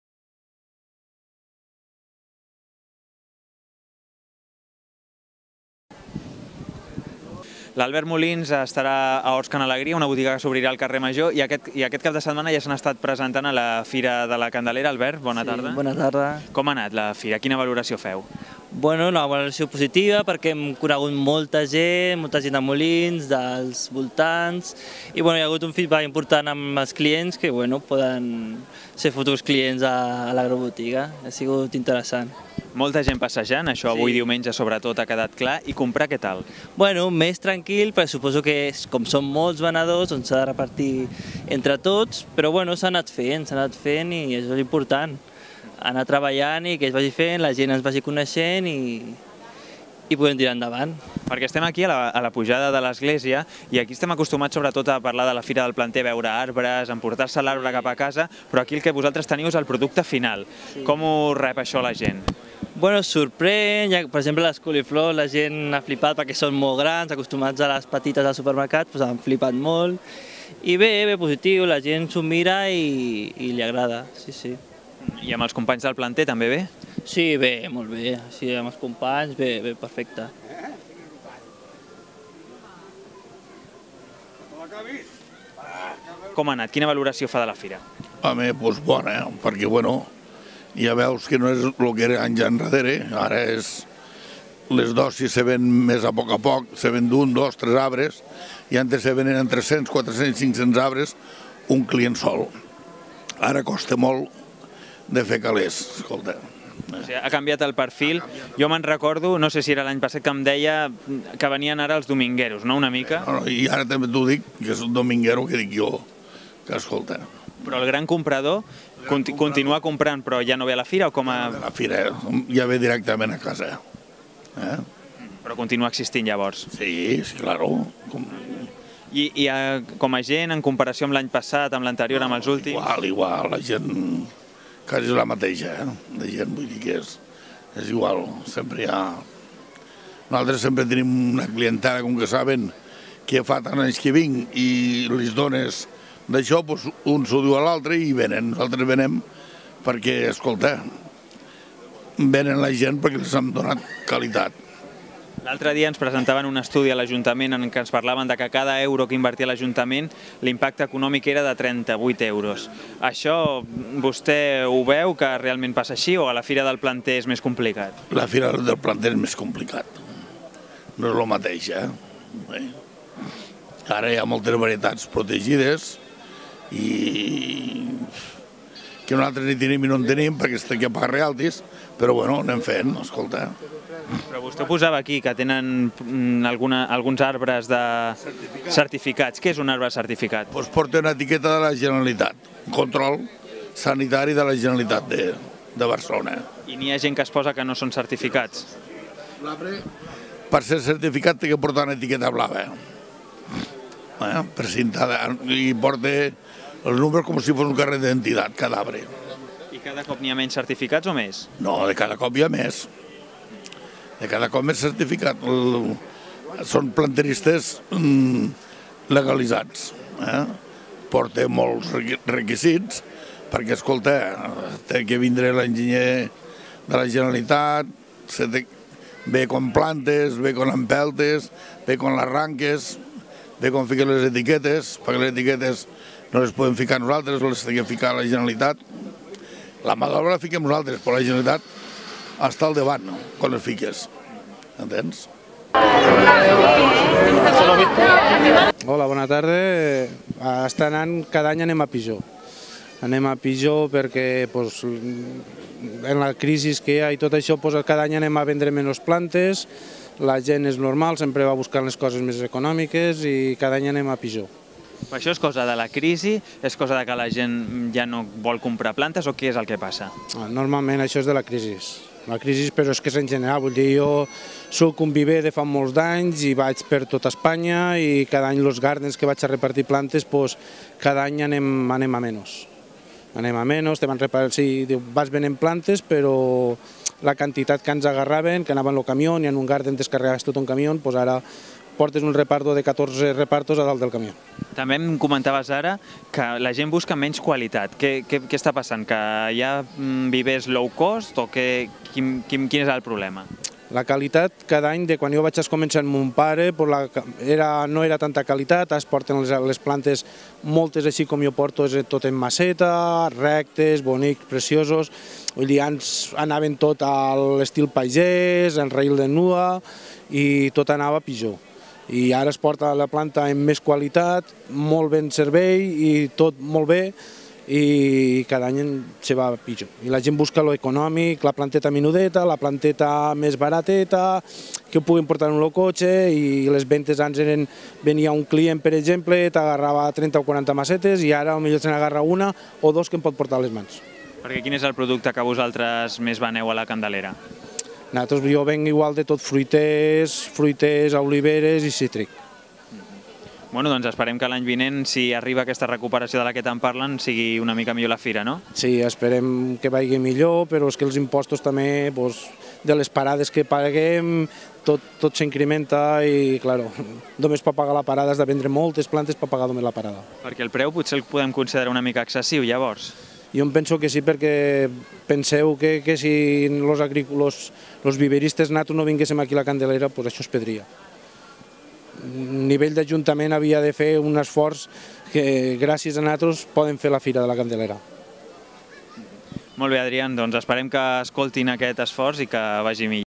Fira de planter de la 163a Fira de la Candelera
Reportatge de Viu Molins de Rei sobre la fira de planter de la 163a Fira de la Candelera.